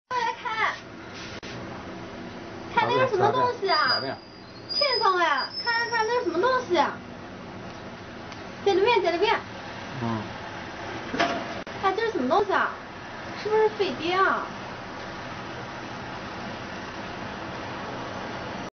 Ovni en Chine ( Ufo in China)
Filmé le 17 août 2006, une vidéo très intéressante, j'attend de voir vos avis avant de dire quoi que ce soit, cependant vidéo à voir absolument!
(Filmed on 17 August 2006, a very interesting video, waiting to see your opinion before saying anything, however, must see video!)